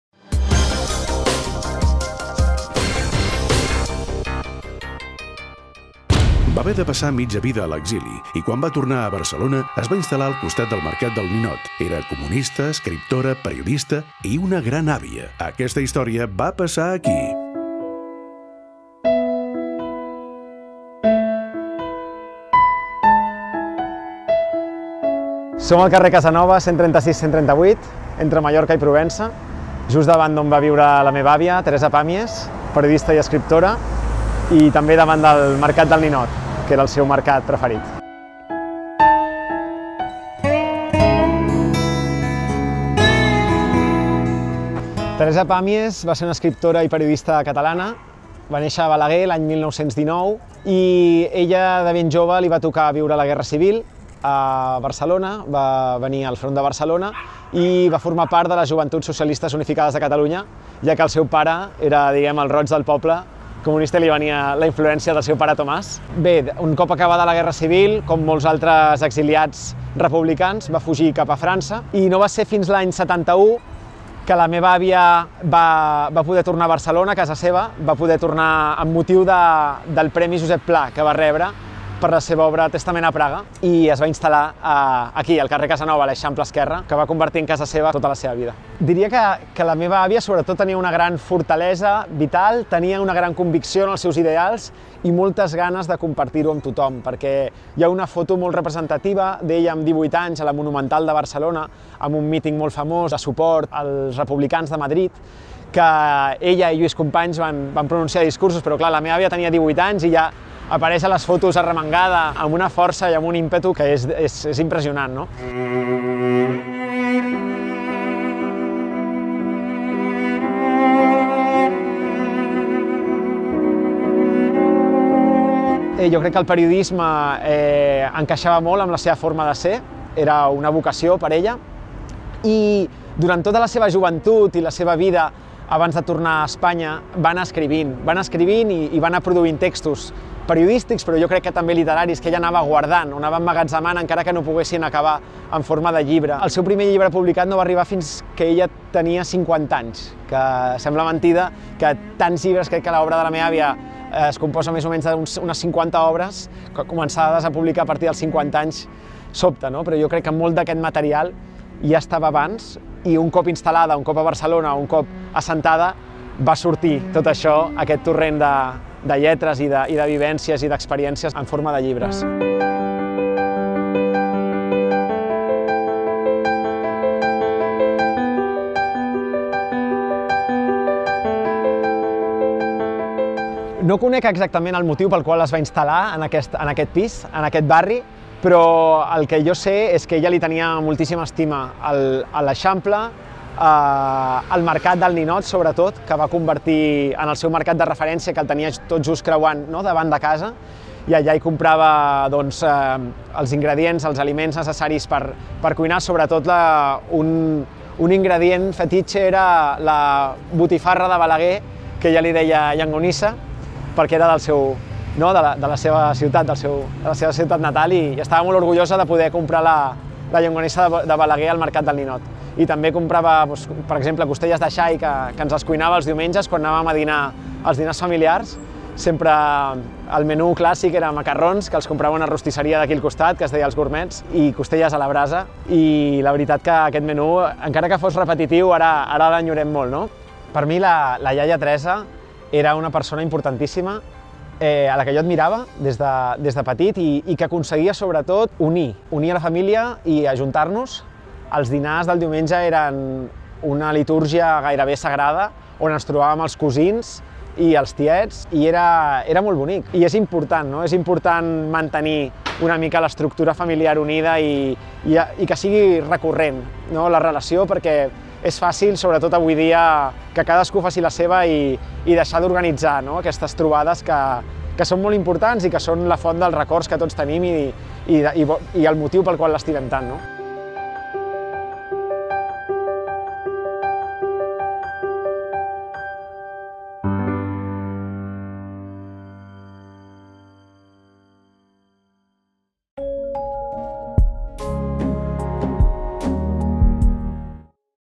Teresa Pàmies, feminista i militant comunista | betevé
Teresa Pàmies va haver de passar mitja vida a l’exili i quan va tornar a Barcelona es va instal·lar al costat del Mercat del Ninot, al carrer de Casanovas, 136-138. L’escriptora i periodista Teresa Pàmies Bertran va néixer a Balaguer l’any 1919 i de ben jove li va tocar viure la Guerra Civil al front de Barcelona i va formar part de les Joventuts Socialistes Unificades de Catalunya.

Primera obra als 50 anys
Àlex Pàmies, net de Teresa Pàmies, ens explica que la seva àvia va exiliar-se a França, la República Dominicana, Cuba i Mèxic, on va estudiar periodisme a la Universitat Femenina. Pàmies va tornar a Barcelona l’any 1971 per rebre el Premi Josep Pla per la seva obra ‘Testament a Praga’ i ja s’hi va quedar a viure.

El primer llibre de Teresa Pàmies no es va publicar fins que va tenir 50 anys, però bona part de la seva obra ja s’havia incubat anys abans, a través d’escrits i reflexions que Teresa Pàmies feia com una mena de teràpia vital. Teresa Pàmies va morir el 13 de març del 2012 a Granada amb un llegat d’una cinquantena de llibres i el record del veïnat i comerciants del Mercat del Ninot, la seva segona casa.